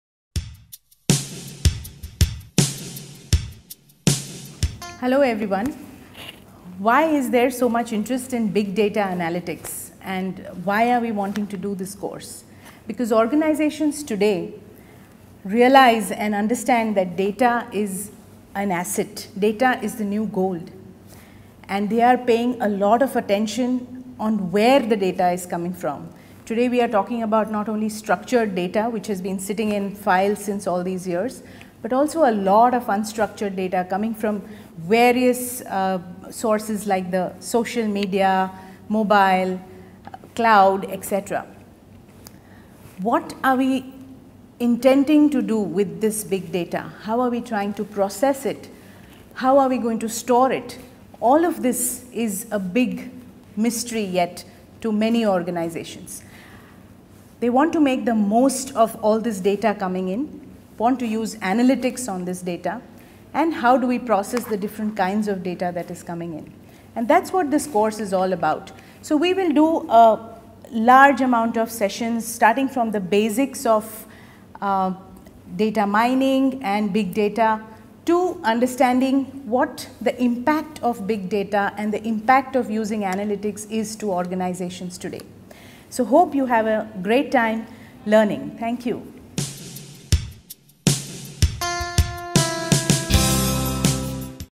Big Data Analytics at SPJIMR
Prof. Aditi Divatia speaks the importance of Big Data and how the course will help the aspirants